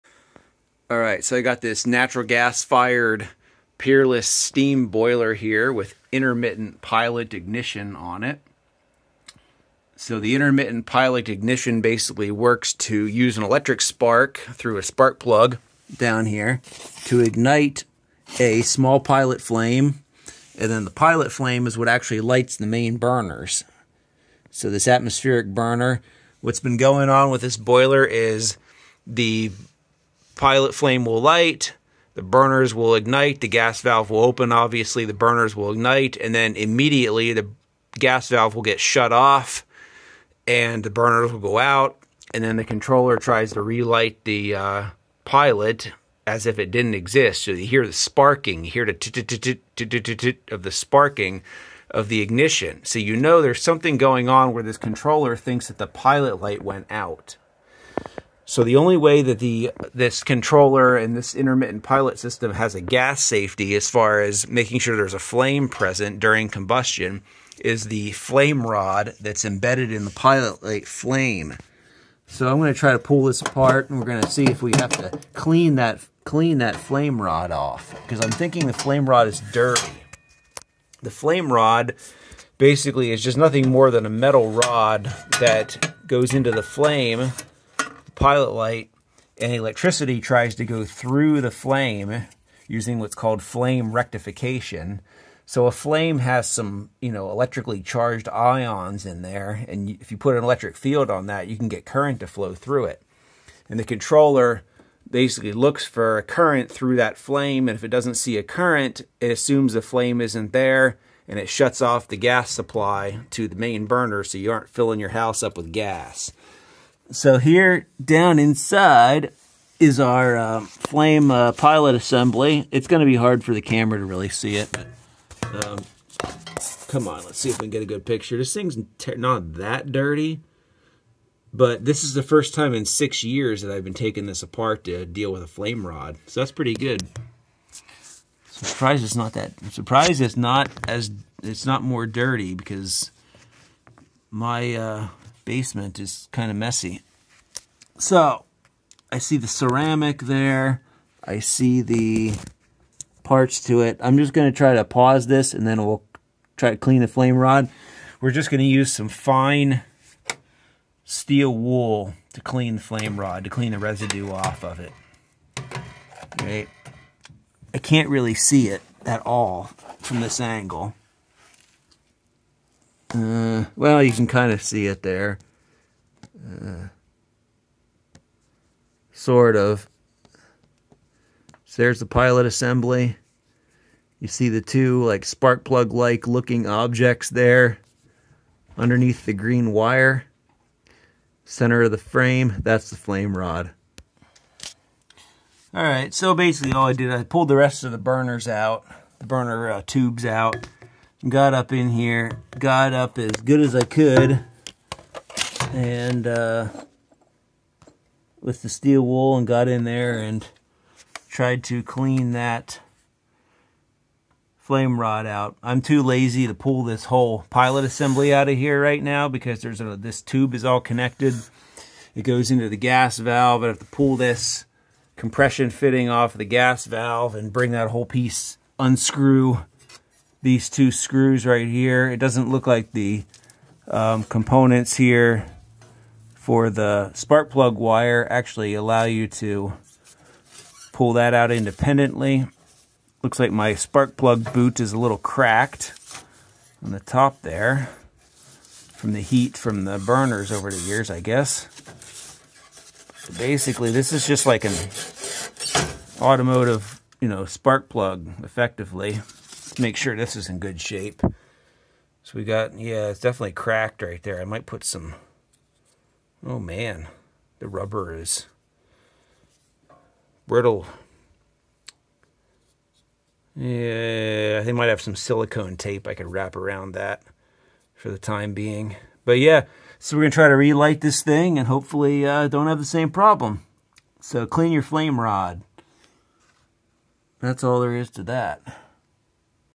Dirty Flame Rod in Intermittent-Pilot Ignition Boiler
This intermittent-pilot boiler had troublesome ignition - it would light, then go out (gas valve shut off), then try to re-light (sparking). On the second attempt, after heating from the pilot, it would stay lit. It turns out dirt on the flame rod was the problem.